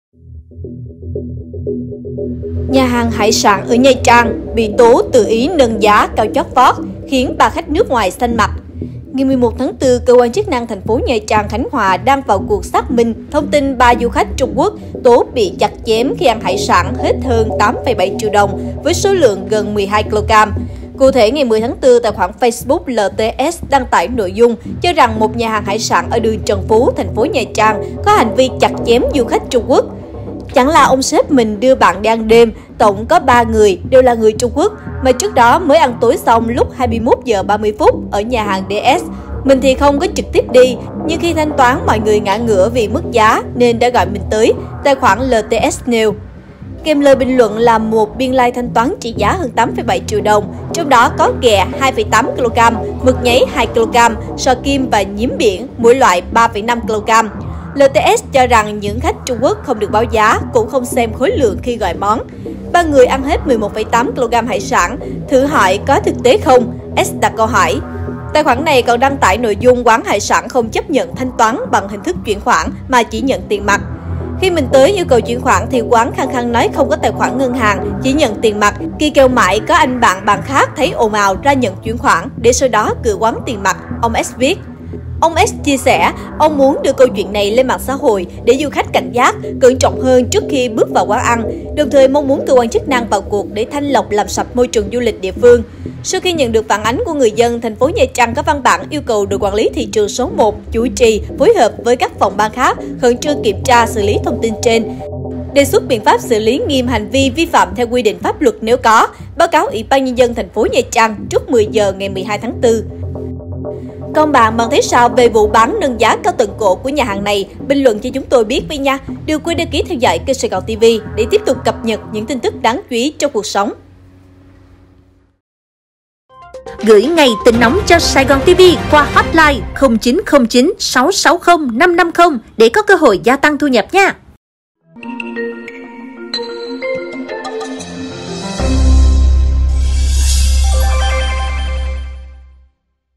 Nhà hàng hải sản BỊ TỐ TỰ Ý NÂNG GIÁ "CAO CHÓT VÓT" khiến 3 khách T.Quốc XANH MẶT
Cụ thể ngày, tài khoản facebook L.T.S đăng tải nội dung cho rằng một nhà hàng hải sản ở đường Trần Phú (TP Nha Trang) có hành vi "chặt chém" du khách Trung Quốc.
"Chẳng là ông sếp mình đưa bạn đi ăn đêm, tổng có 3 người đều là người Trung Quốc, mà trước đó mới ăn tối xong lúc 21h30 ở nhà hàng Đ.S. Mình thì không có trực tiếp đi, nhưng khi thanh toán mọi người ngã ngửa vì mức giá nên đã gọi mình tới" - tài khoản L.T.S nêu.

Kèm lời bình là một biên lai thanh toán trị giá hơn 8,7 triệu đồng, trong đó có ghẹ 2,8kg, mực nháy 2kg, sò kim và nhím biển mỗi loại 3,5kg. L.T.S cho rằng những khách Trung Quốc không được báo giá, cũng không xem khối lượng khi gọi món.

"3 người ăn hết 11,8kg hải sản. Thử hỏi có thực tế không?" - S. đặt câu hỏi.

Tài khoản này còn đăng tải nội dung quán hải sản không chấp nhận thanh toán bằng hình thức chuyển khoản, mà chỉ nhận tiền mặt.

/ Theo báo Dân Trí & Video tổng hợp /